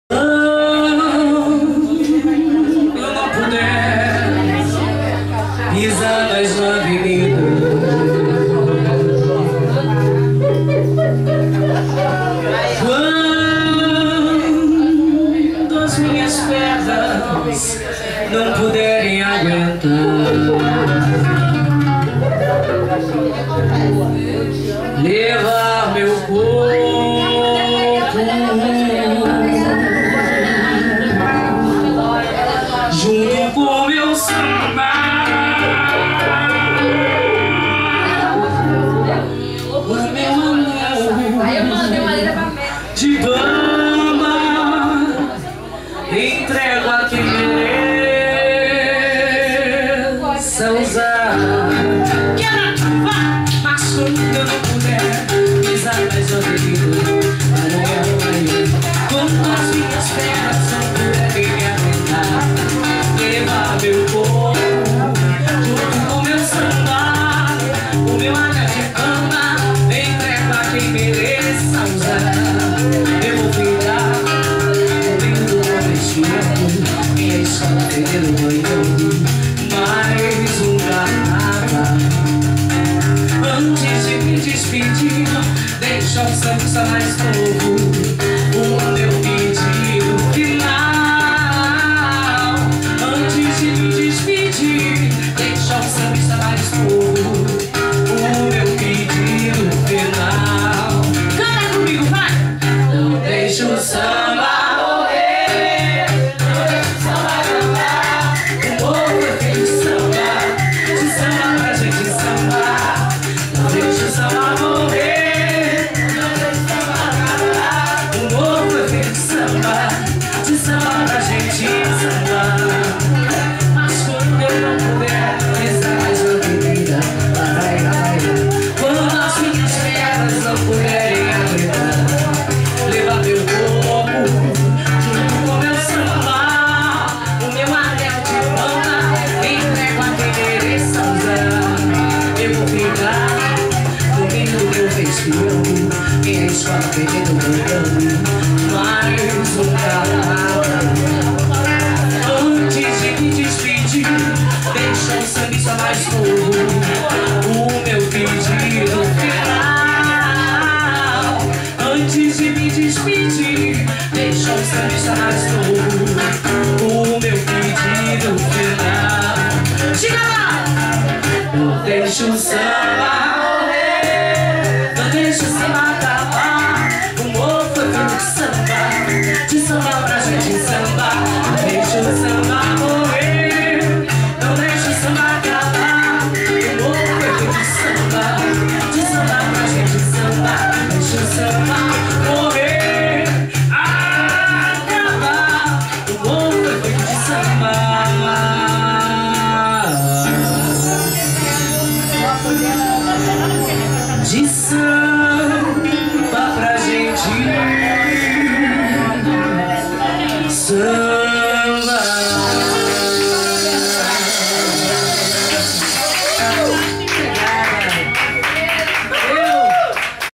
Samba com Juliana Farina @ Musique Lounge 1/12
1º Parte do show de samba de Juliana Farina no Projeto Verão do Musique Lounge. dia 17/01/2010